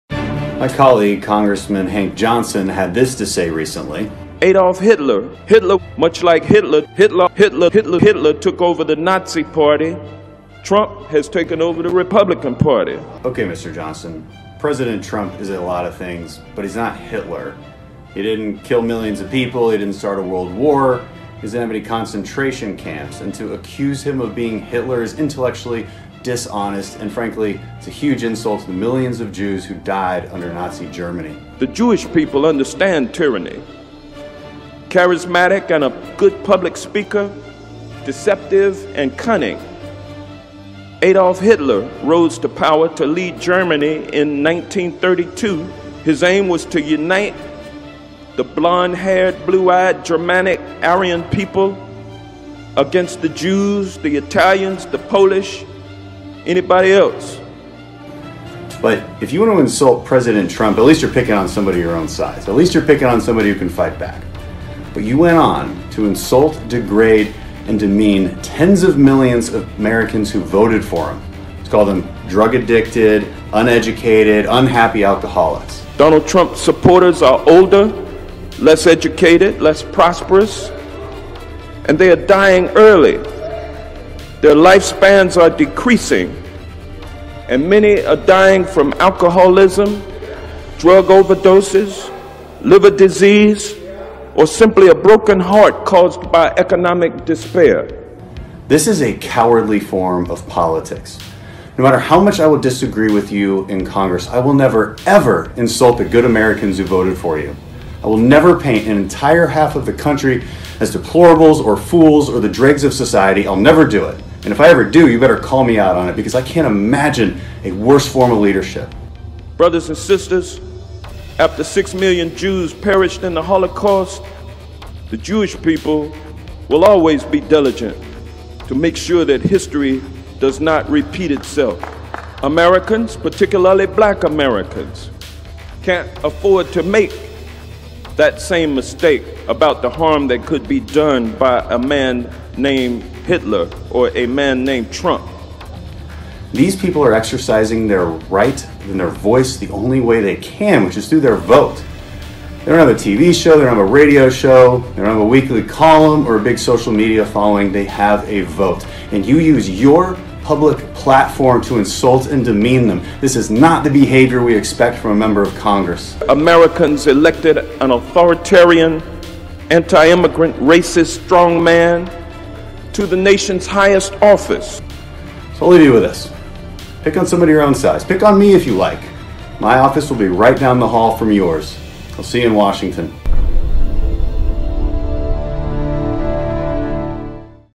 Crenshaw Calls Out Hank Johnson For Comparing Trump To Hitler
After Democrat Congressman Compares Trump To Hitler War Hero Congressman Steps Up To Call Him Out